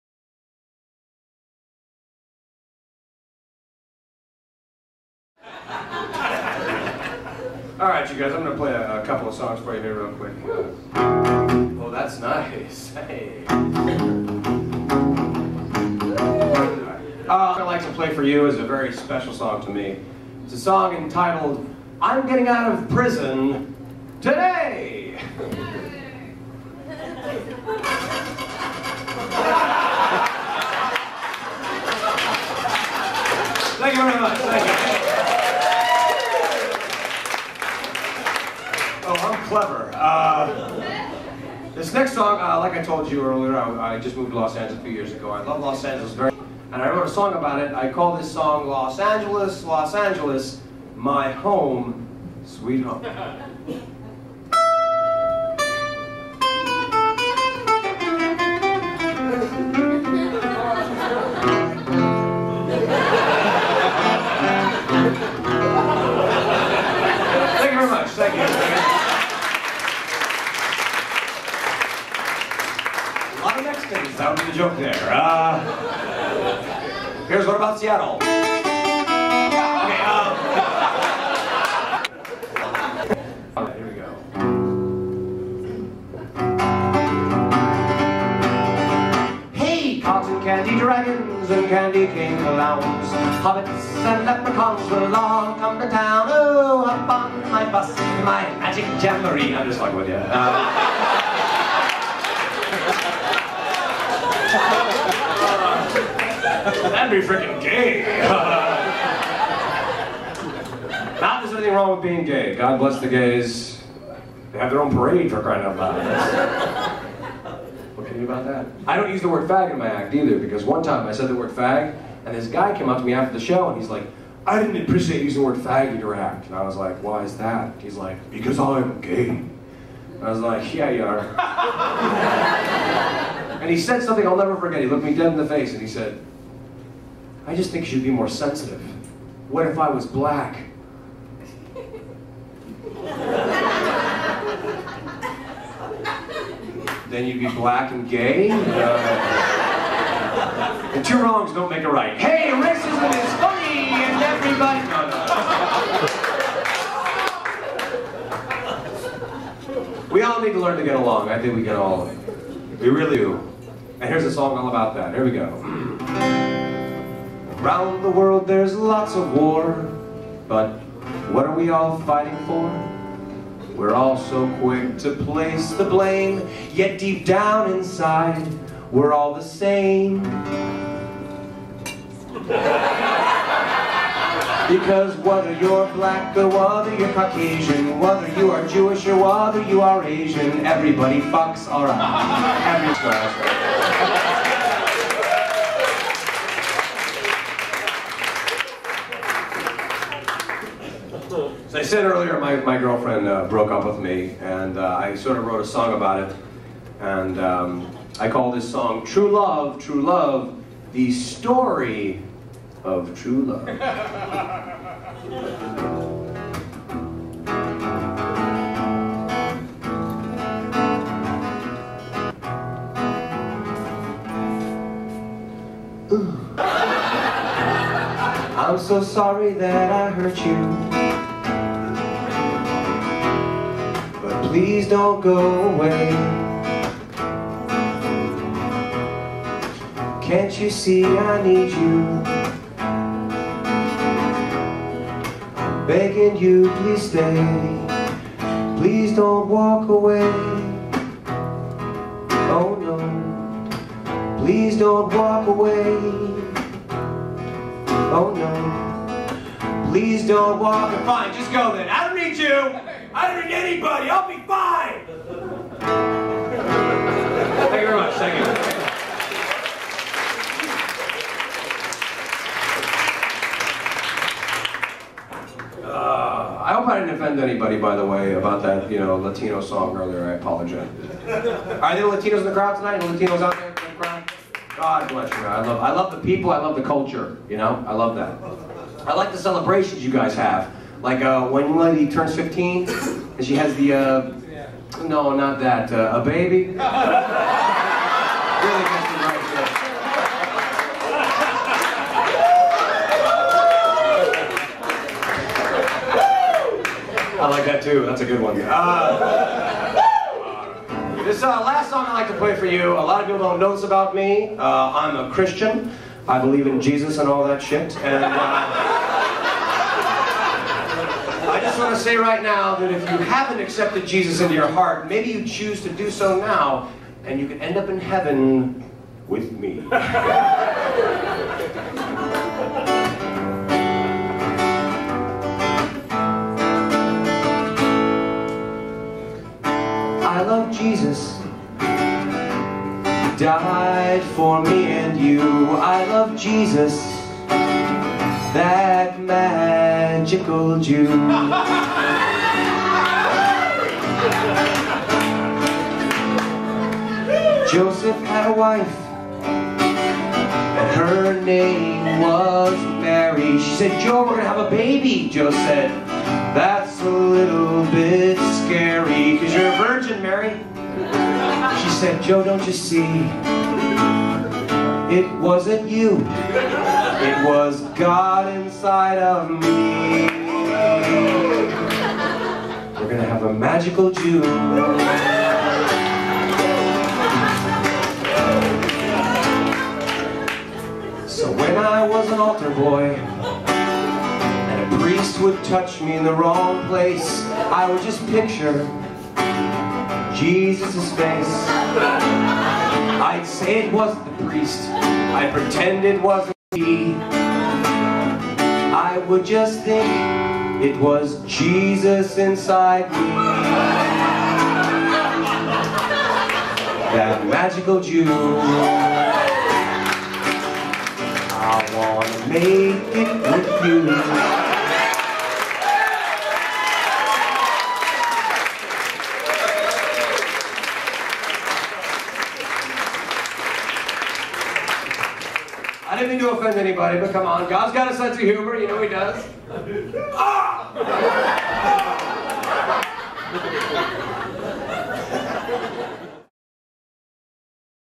Rick Kunkler with Guitar
Rick Kunkler with Guitar, at the Improv. Funny!